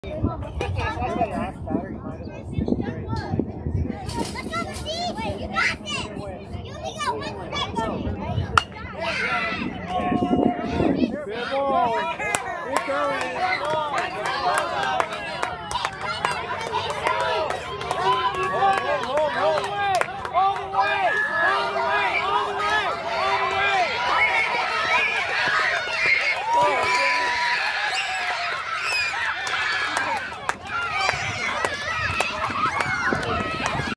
2024.03.26 Vasin Walk-Off Homerun
Top of 5th Inning. No Outs. Two On. Walk Of In-the-Park Homerun.